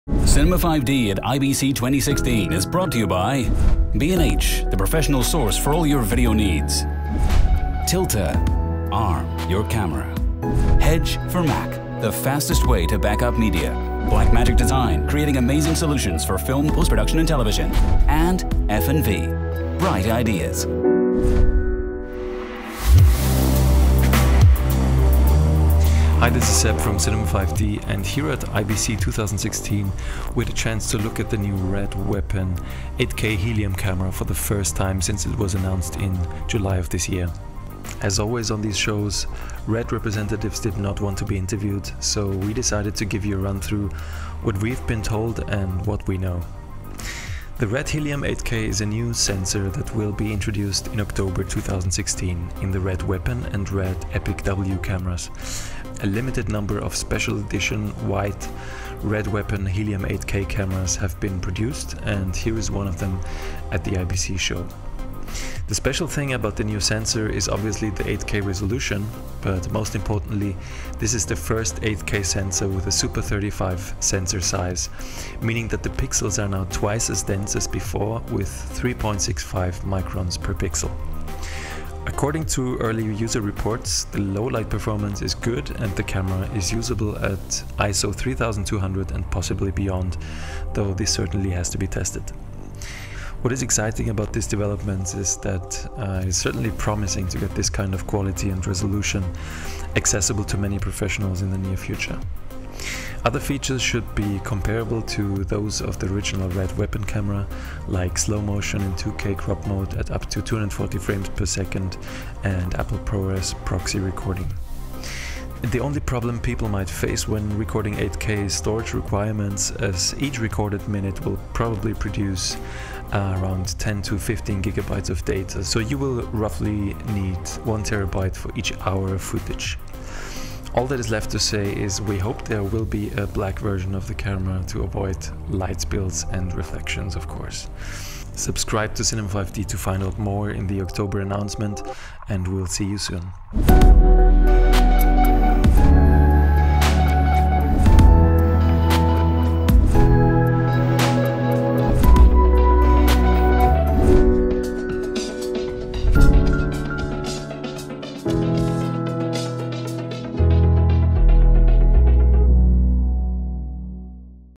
First Hands On With RED Helium 8K Camera
At IBC 2016 we looked at the new RED Weapon Helium 8K camera for the first time. Here's a rundown of all we know for the upcoming October announcement.